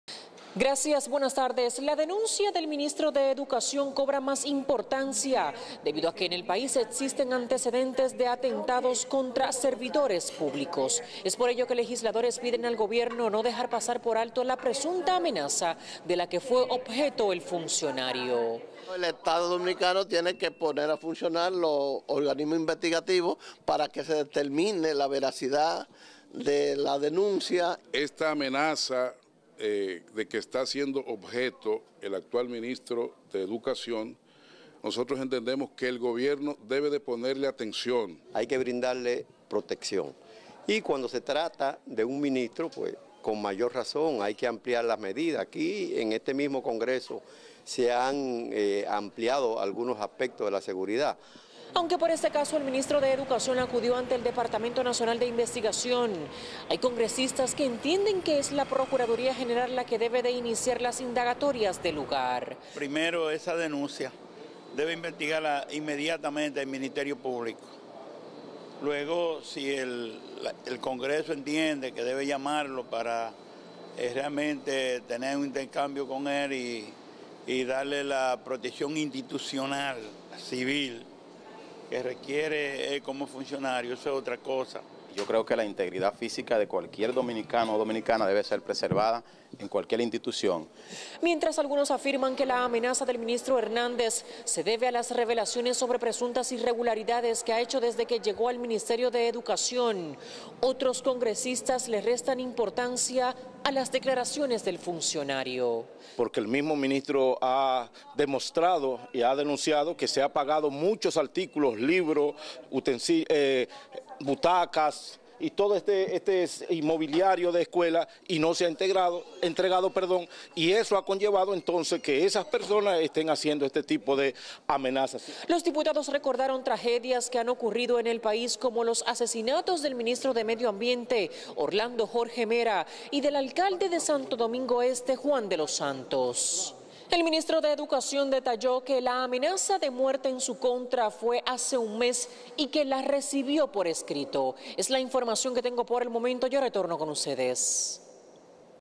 Piden garantizar seguridad del ministro de Educación tras denuncia de amenazas de muerte
Ante la denuncia de amenaza de muerte que supuestamente recibió el ministro de Educación, Ángel Hernández, legisladores de distintos partidos llamaron al gobierno a movilizar de inmediato los organismos de investigación y de seguridad para investigar el caso y garantizar la integridad física del funcionario.